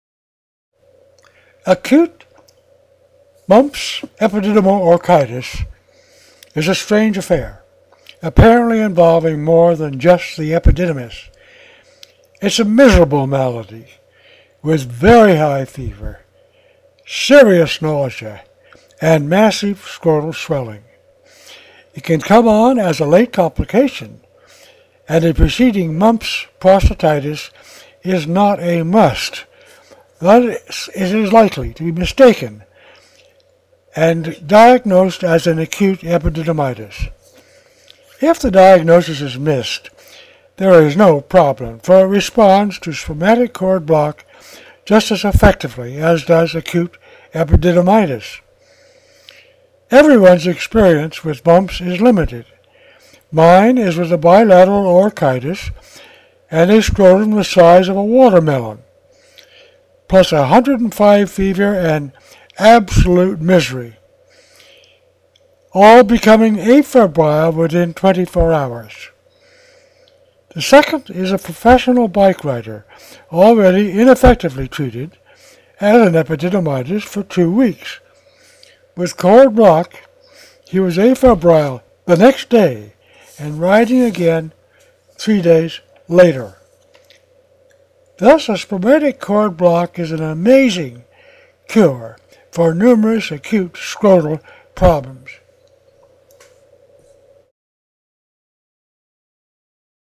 Mumps Epididymo-orchitis. Its prompt resolutiom
The complication of Mumps in the scrotum is a serious setback after the Parotitiis. it responds as does Acute Epididiymitis, patient promptly afebrile and fully active in 72 hours.It  is the spermatic cord block that accomplishes this, and consistently.